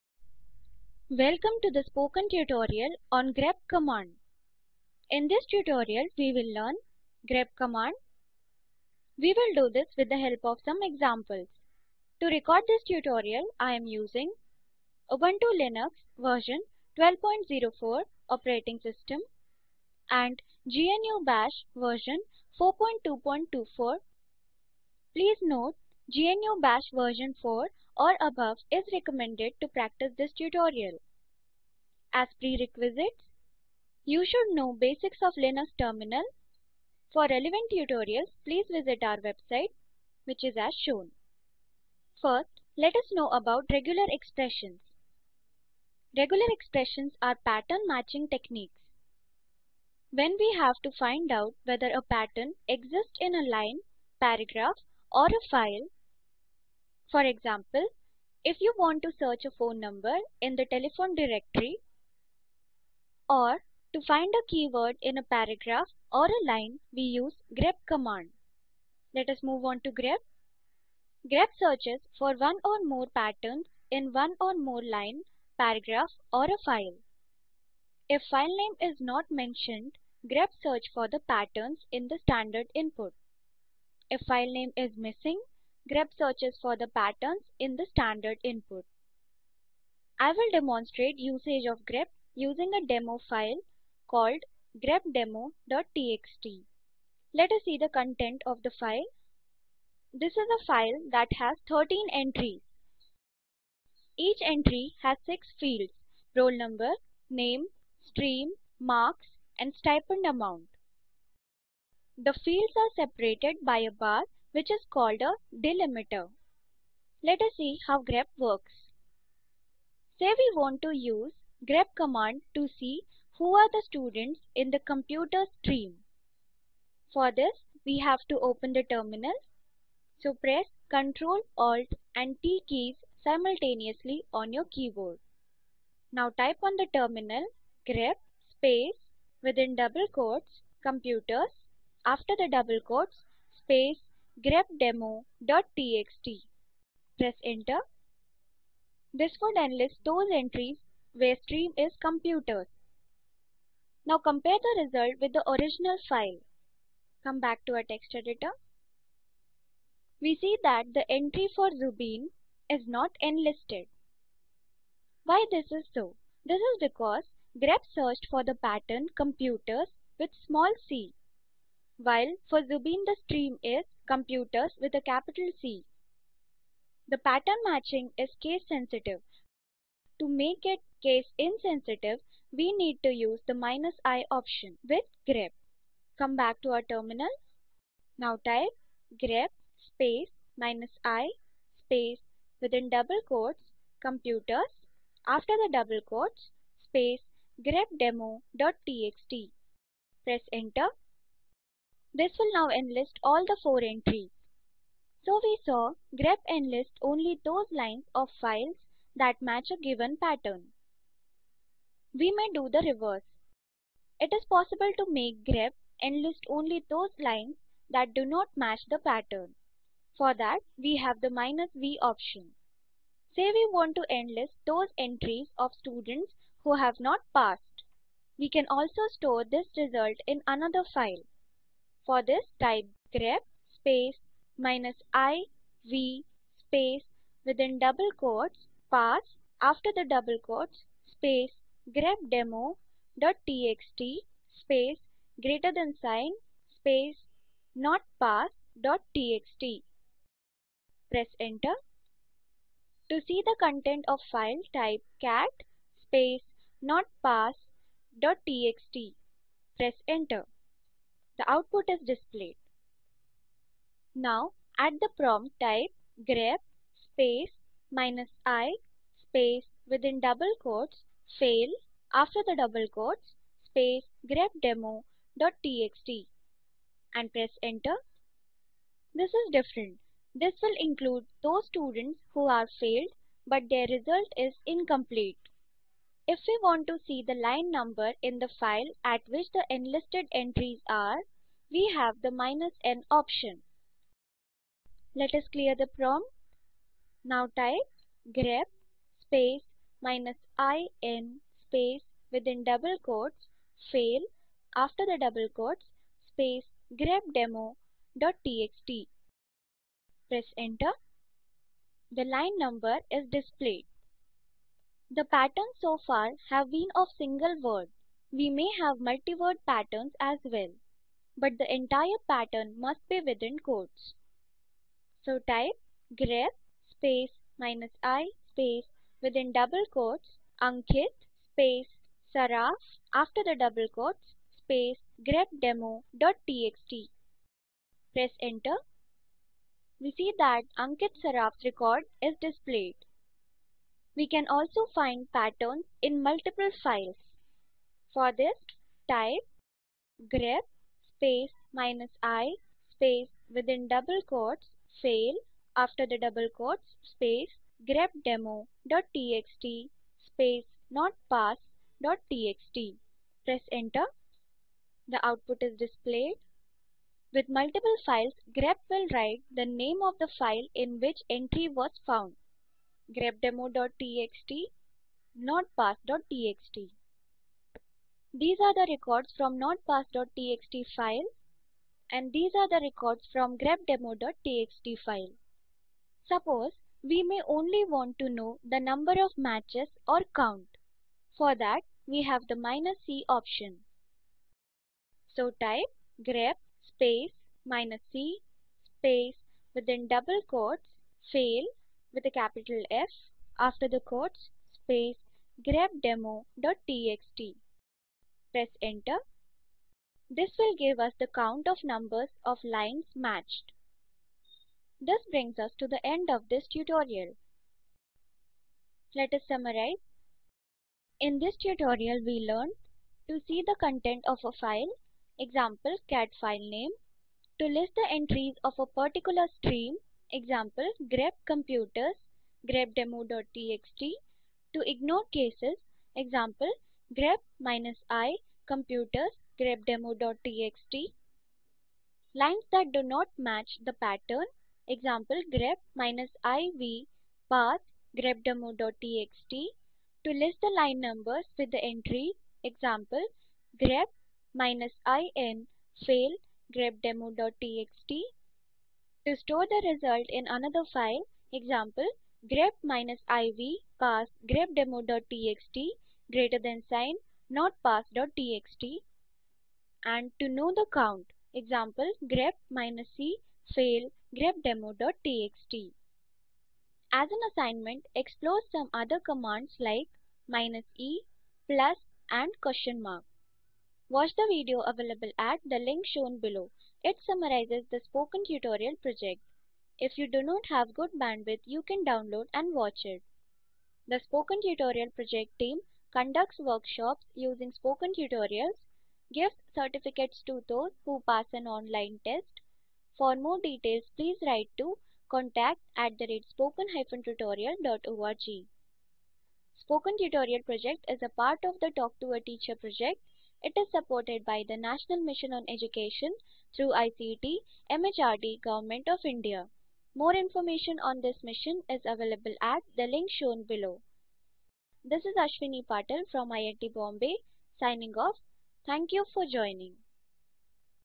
The grep command - English
The grep command

    To see the content of a file
    To list the entries of a particular stream
    To ignore cases
    Lines that do not match the pattern
    To list the line numbers with the entries
    To store the result in another file
    To know the count